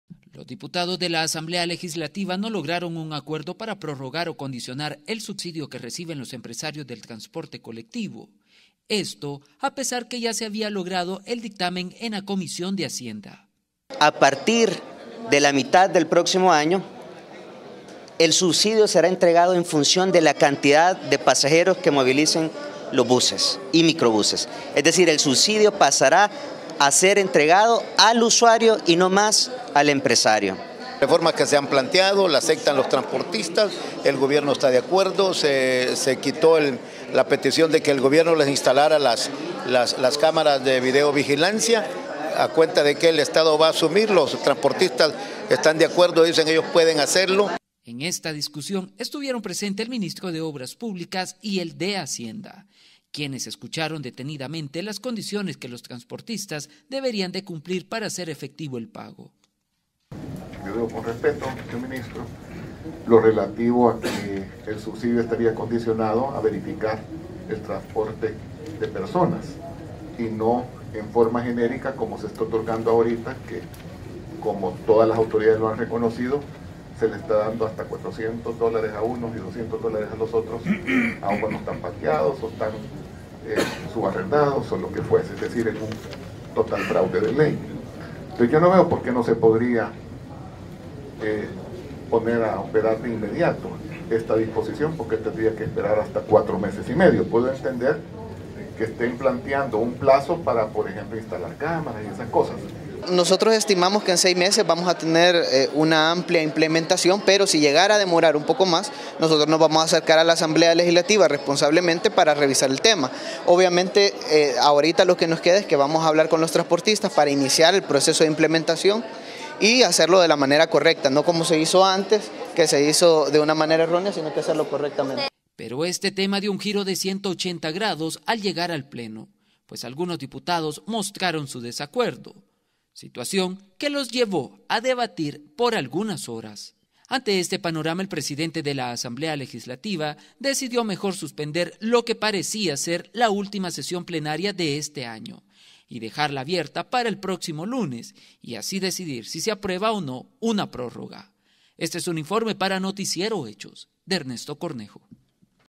Subsidio a transportistas se mantiene por 6 meses, luego se lo condicionarán
En seis meses, los transportistas deben equipar sus unidades, contratar seguros y mejorar las condiciones laborales para conservar el subsidios a las unidades.